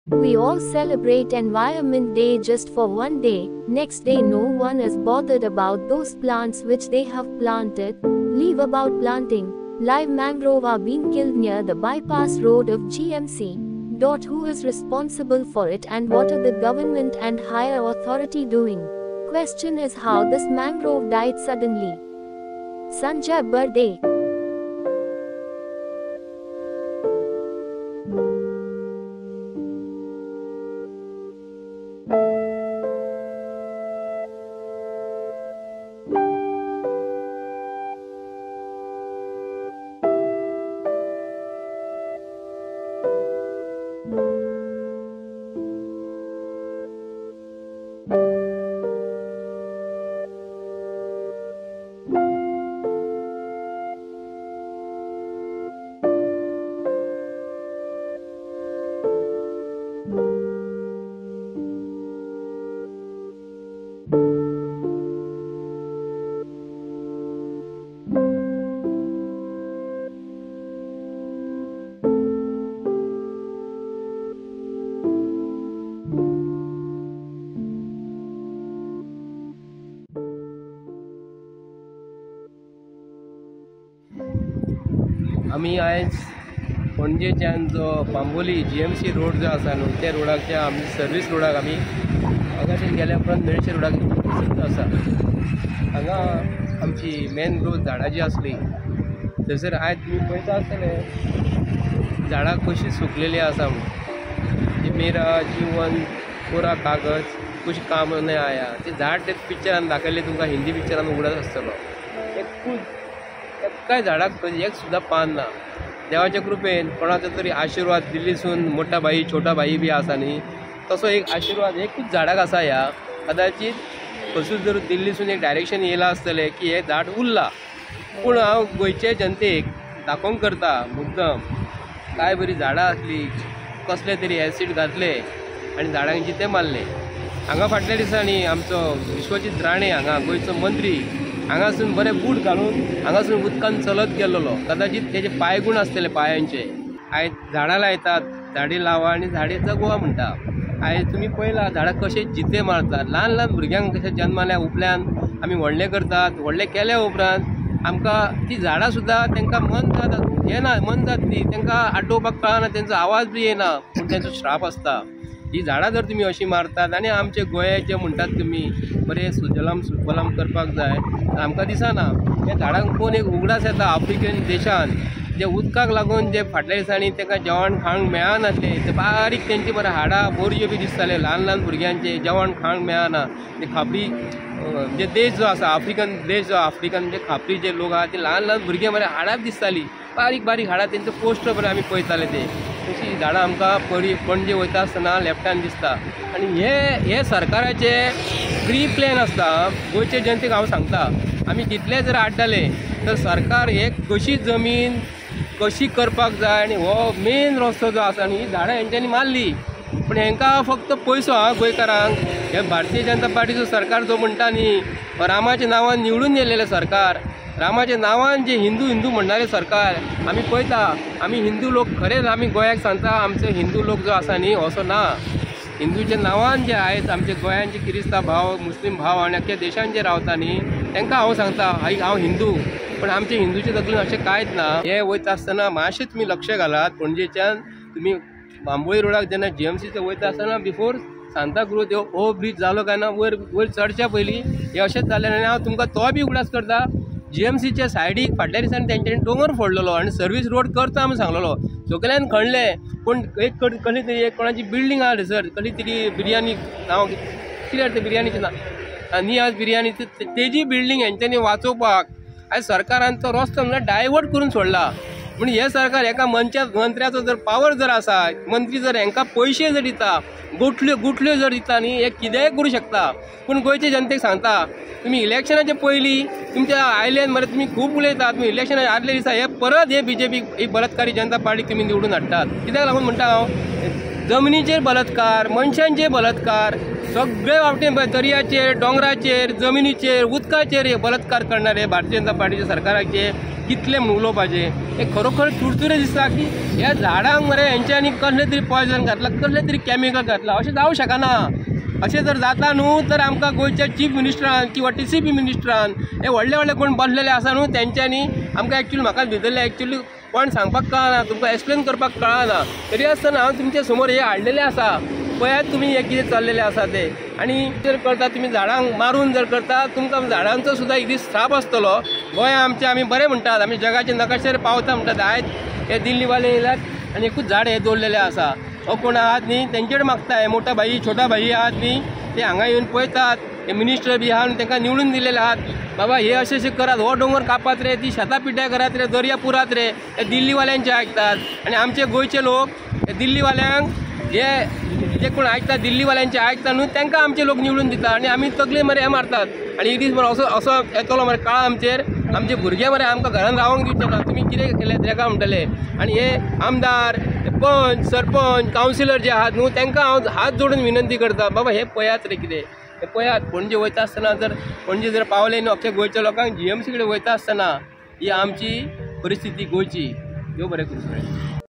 Is this destruction taking place to make Chota and mota bhai happy ??? Sanjay Barde
We all celebrate environment day just for one day, next day no one is bother about those plants which they have planted..leave about planting, live mangrove are been killed near bypass road of GMC..who is responsible for it and what the government and higher authority doing..question is how this mangrove died suddenly??